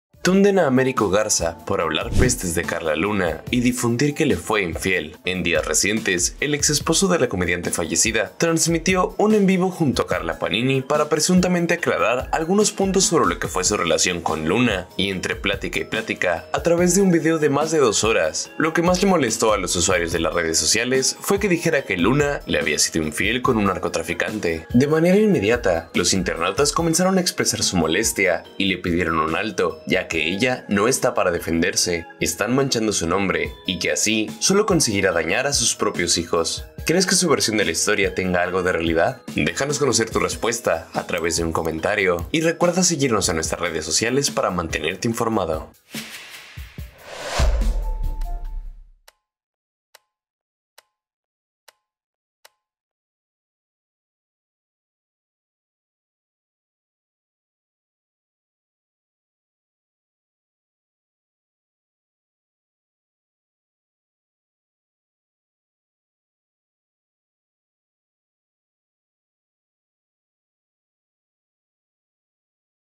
Américo Garza asegura que Karla Luna le fue infiel
En un video reciente el ex de la fallecida Karla Luna, Américo Garza, confesó que la comediante le fue infiel en varias ocasiones, entre ellas con un peligroso capo de la droga y un famoso payaso de la televisión.

En el video, también se encontraba la actual pareja del empresario, Karla Panini, y las críticas en contra de ambos no se han hecho esperar.

"Ella ya no está para defenderse", y “¡Qué poco hombre resultó el ex de Karla Luna!, fueron algunos de los comentarios en contra de Américo Garza.